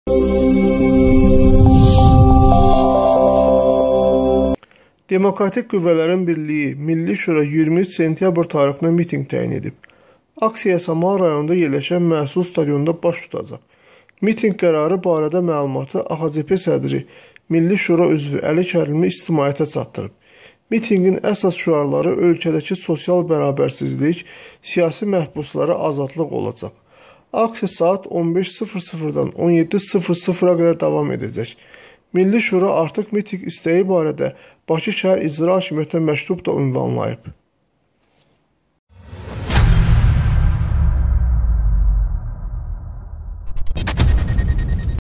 Milli Şura "siyasi məhbuslara azadlıq!" tələbi ilə mitinq təyin etdi
Demokratik qüvvələrin birliyi Milli Şura 23 sentyabr tarixinə mitinq təyin edib. Aksiya Yasamal Rayonunda yerləşən Məhsul stadionunda baş tutacaq. Mitinq qərarı barədə məlumatı AXCP sədri, Milla Şura üzvü Əli Kərimli ictimaiyyətə çatdırıb. Mitinqin əsas şüarları Ölkədəki sosial bərəbərsizlik , siyasi məhbuslara azadlıq olacaq.  Aksiya saat 15-00 dan 17 00-a qədər davam edəcək. Milli Şura artıq mitinq istəyi ilə bağlı Bakı Şəhəri İcra Hakimiyyətinə məktub da ünvanlayıb.